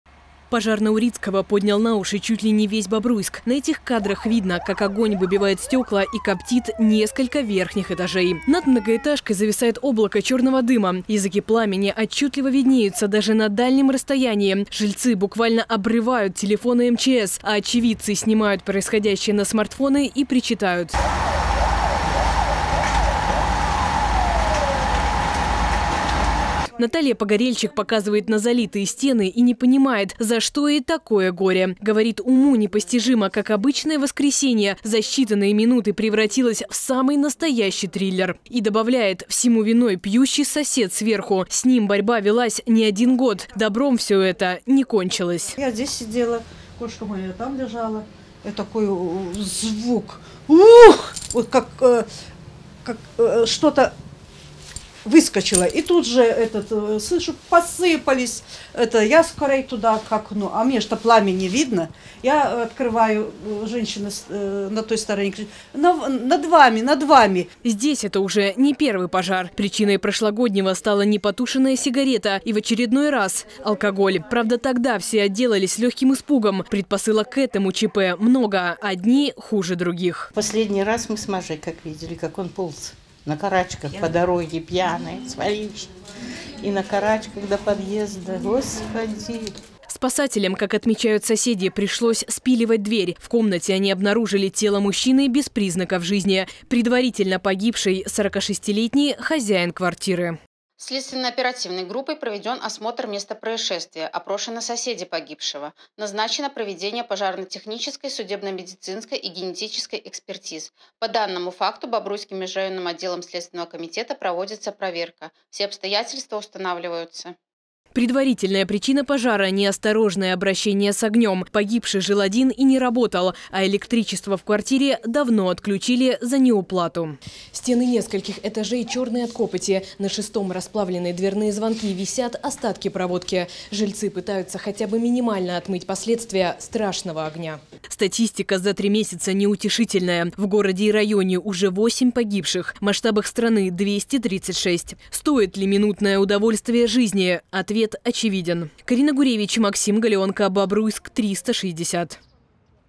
Подробности огненной трагедии
Накануне на Урицкого произошёл серьезный пожар. В очередной раз неосторожное обращение с огнём закончилось гибелью. Выбитые окна, закопченные стены и расплавленная проводка.